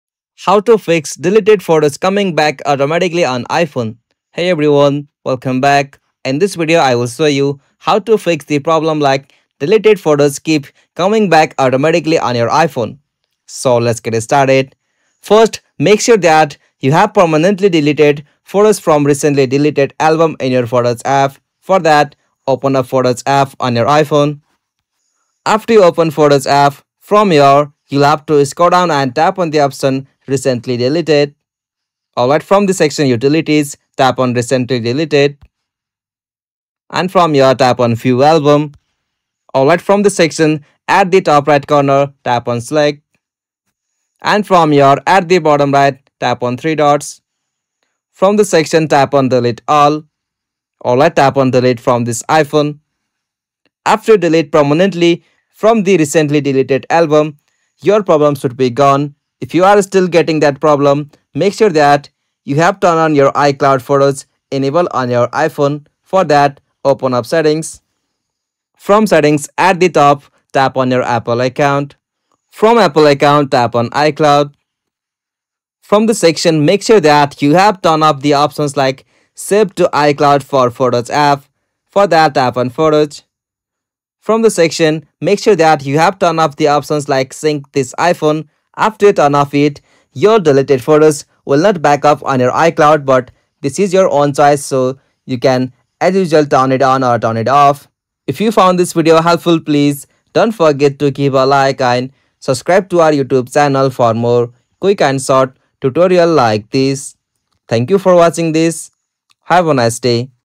How to Fix Deleted Photos Coming Back Automatically on iPhone
Are deleted photos on your iPhone coming back automatically? This video will guide you through the steps to resolve this issue and stop deleted photos from reappearing. Whether you're asking, "Why do my deleted photos keep coming back on iPhone?" or need help with managing your photo storage, this tutorial has you covered. Learn how to fix the issue and make sure your photos stay deleted for good. Watch now to see how you can solve this problem on your iPhone today!

Timestamps:
00:00 How to Fix Deleted Photos Coming Back Automatically on iPhone
00:06 Why Do Deleted Photos Keep Coming Back on iPhone?